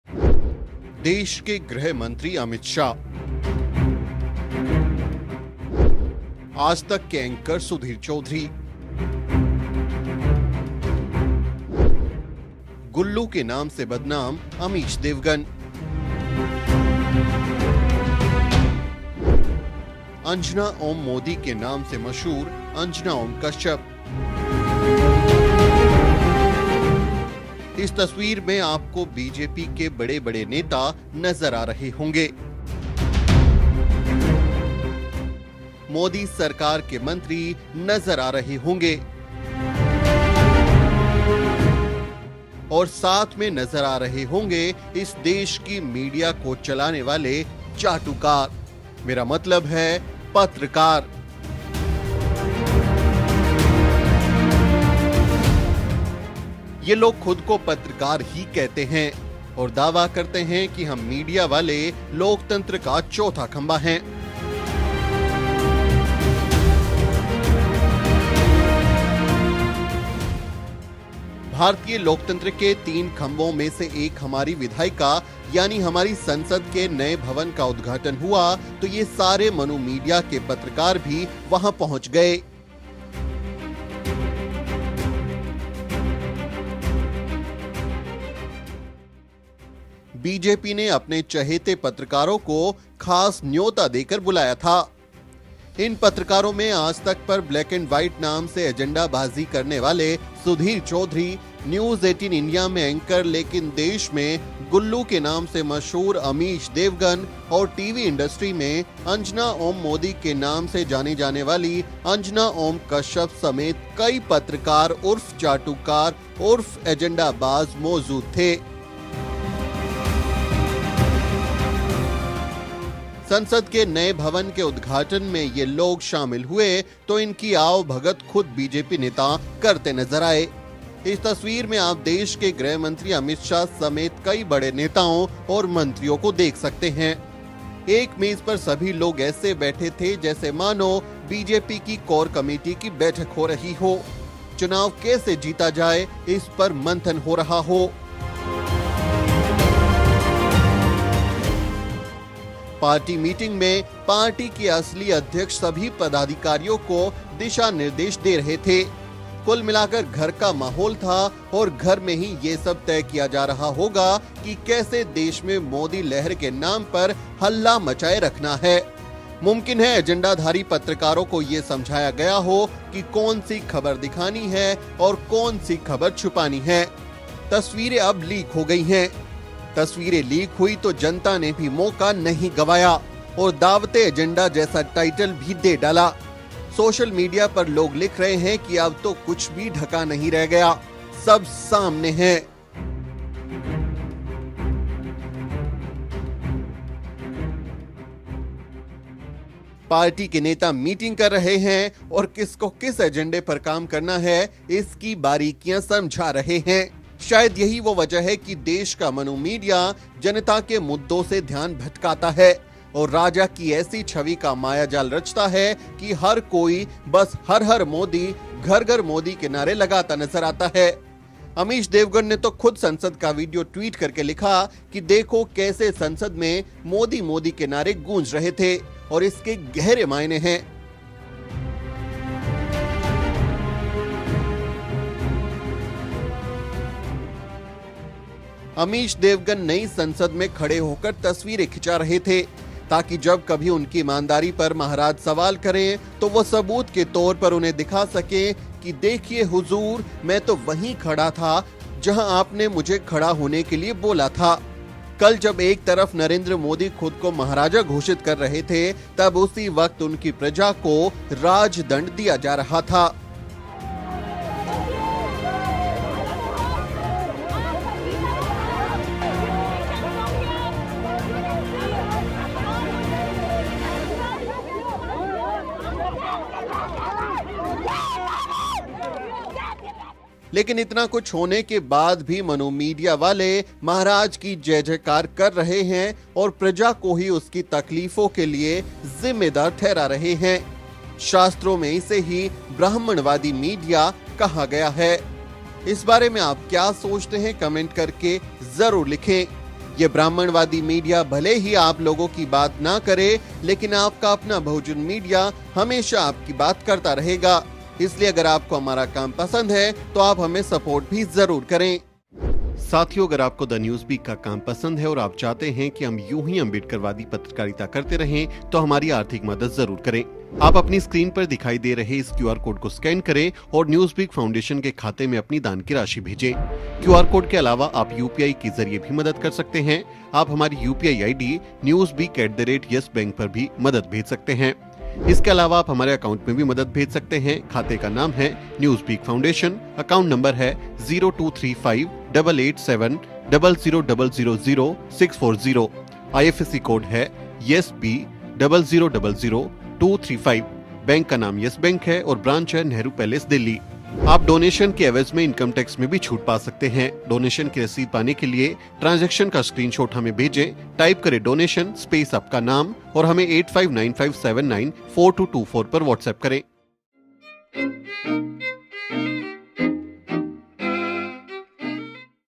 गोदी मीडिया की फोटो लीक, सुधीर चौधरी से लेकर अंजना तक, अमित शाह के साथ मीटिंग में क्या हुआ?
UPI - newsbeak@yesbank